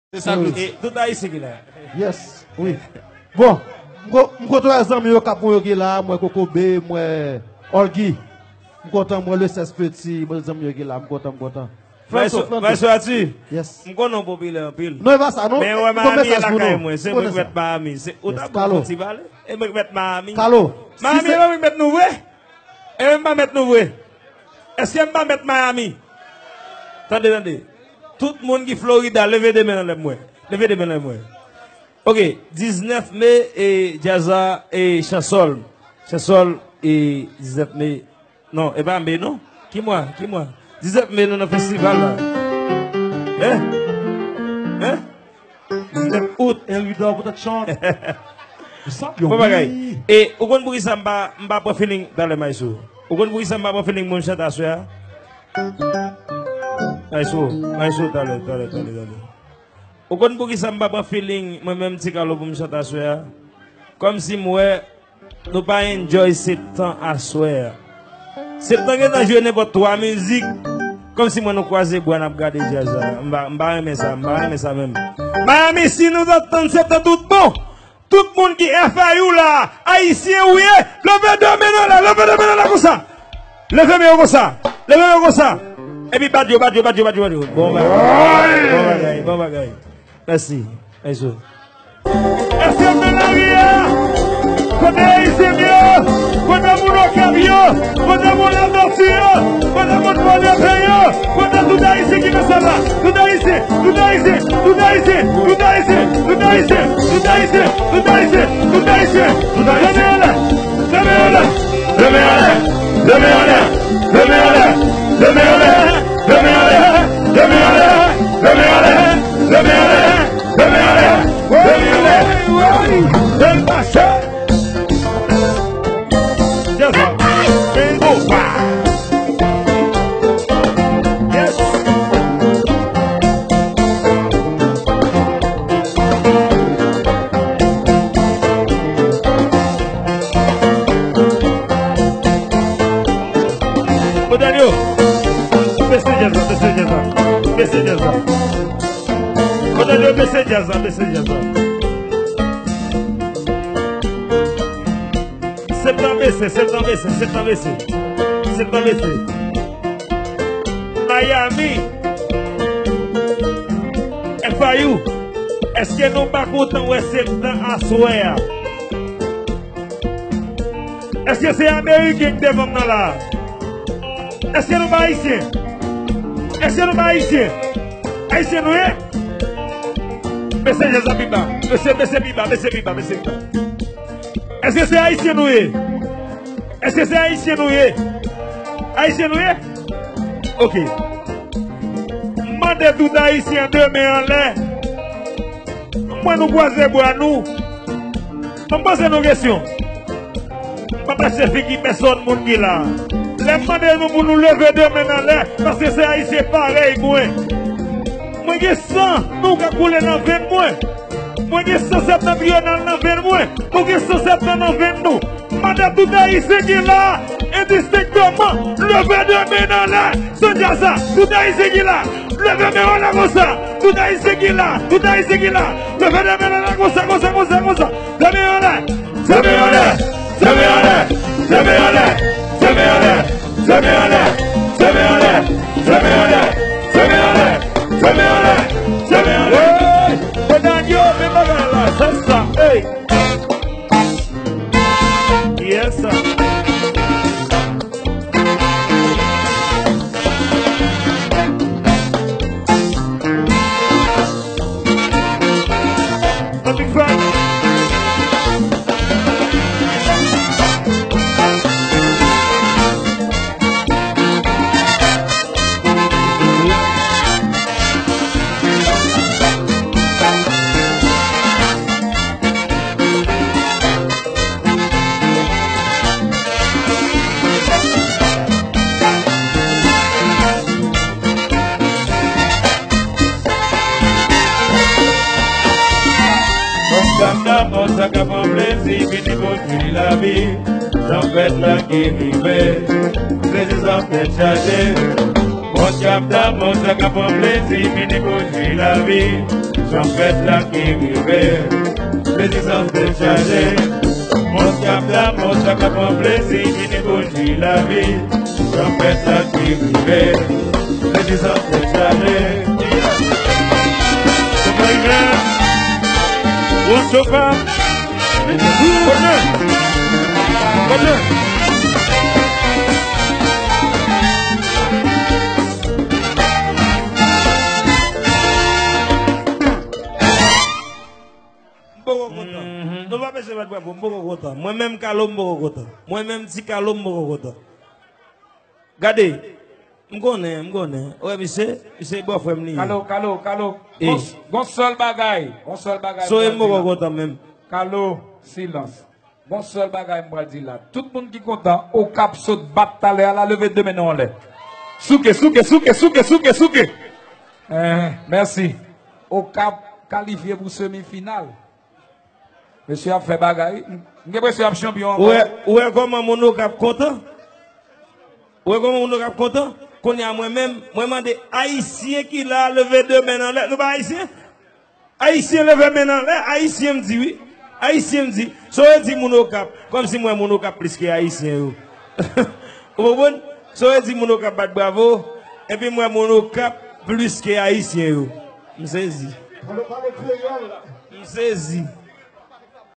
Me rezon kifè yo mete Ti Carlo sou poz nan Septent swiv live Aksion miche sou stage la
Me rezon kifè yo mete Ti Carlo sou poz nan Septent